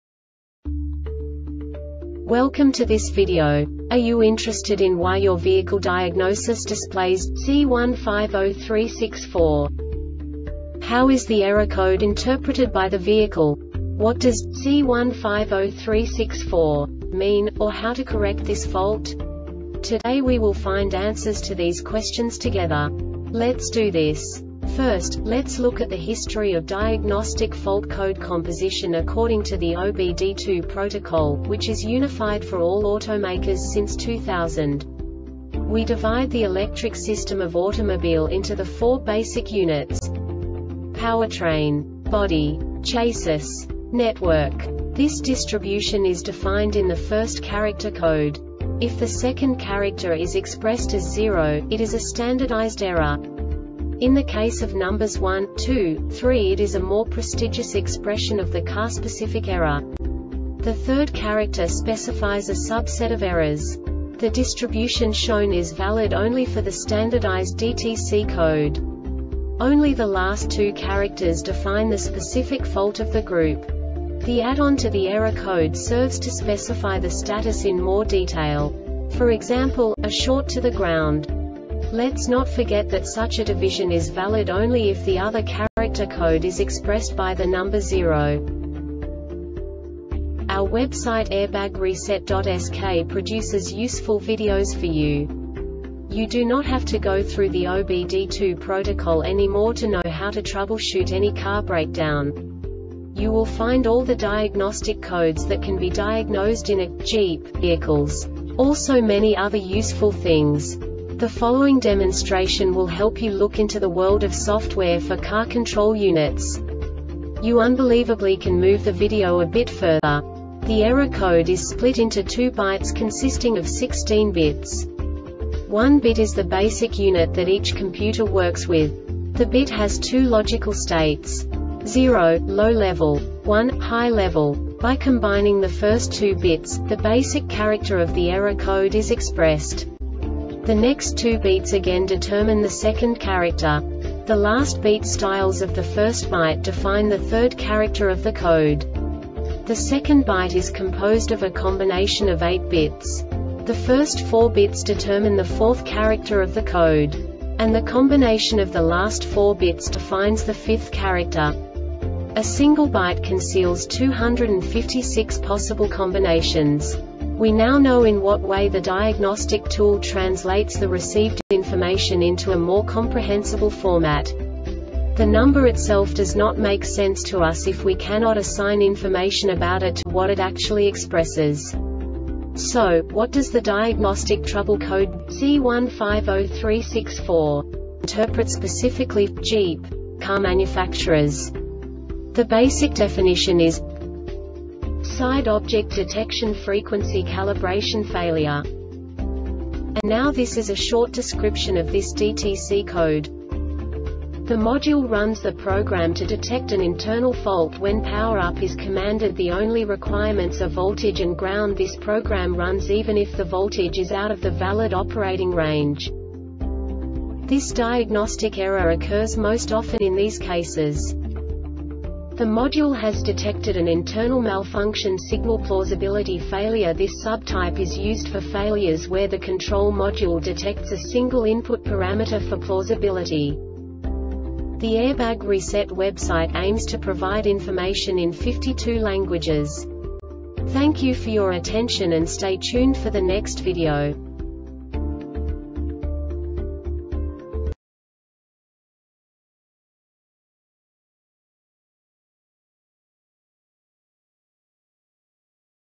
DTC Jeep C1503-64 Short Explanation
Contents:
0:21 Basic DTC analysis according to OBD2 protocol standard.
1:48 Insight into programming
2:58 A diagnostically understandable description of the Diagnostic Trouble Code
3:05 Basic error definition

Make:
Jeep
Code:
C1503 64
Definition:
Side Object Detection Frequency Calibration Failure
Description:
Cause:
The module has detected an internal malfunction.